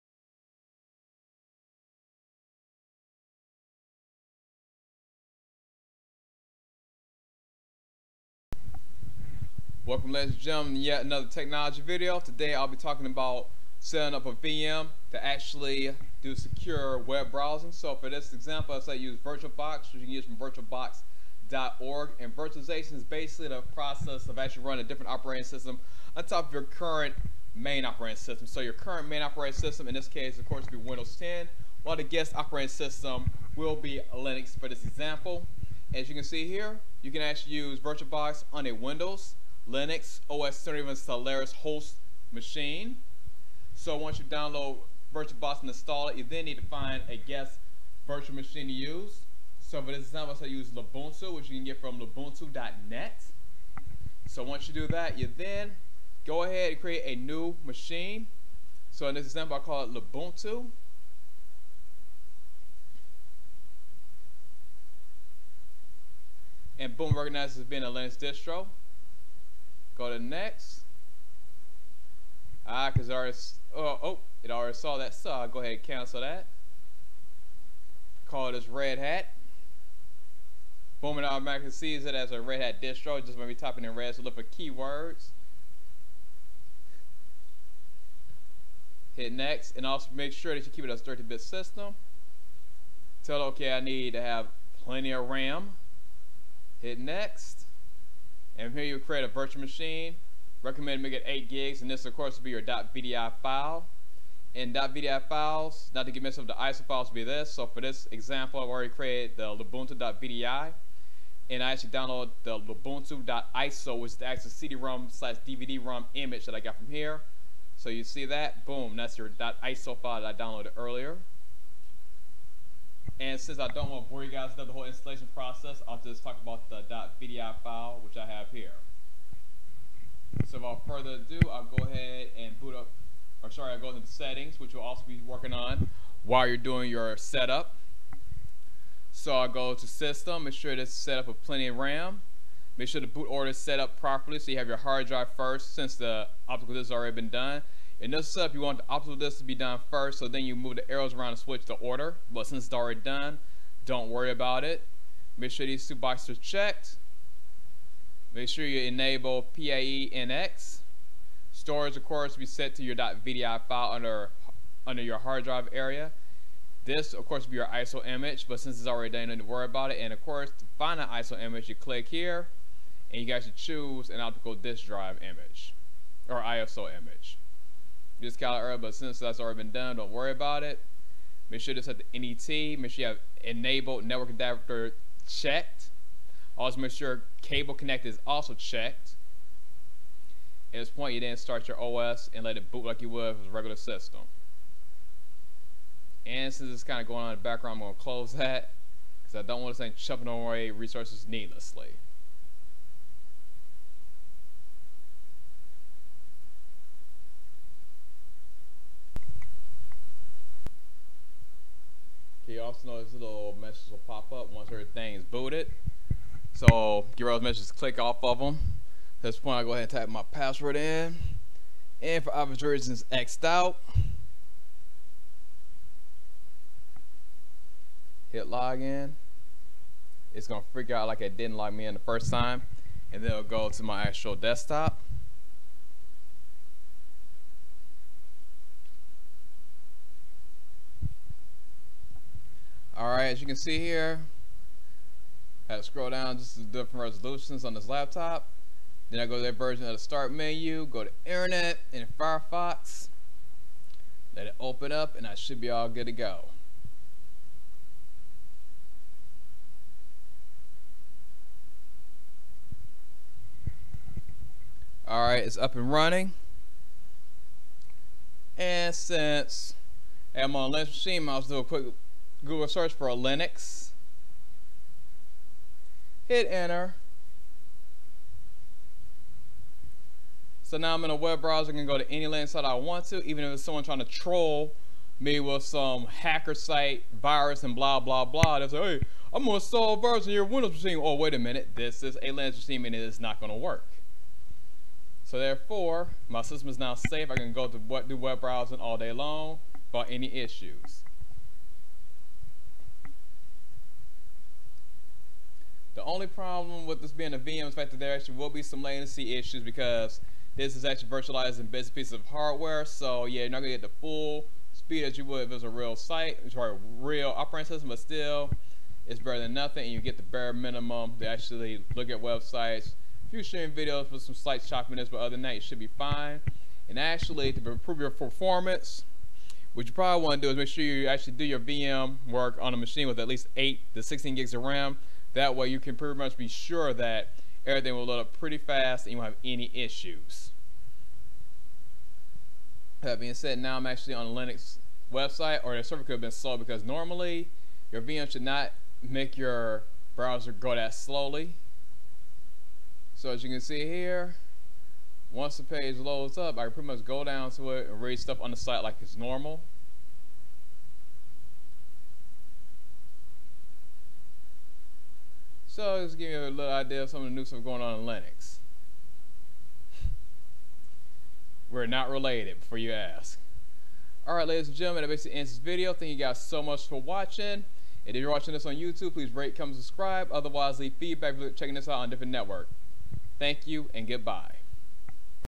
Safe Web Browsing Using Virtualization
A quick tutorial demonstrating how to use Virtualization for safer web browsing.

XBox Live: CulinaryTiger09

Fitness Quiz Bowl 2014 (Runs on Windows, Mac OS X, and Linux with the Java JRE):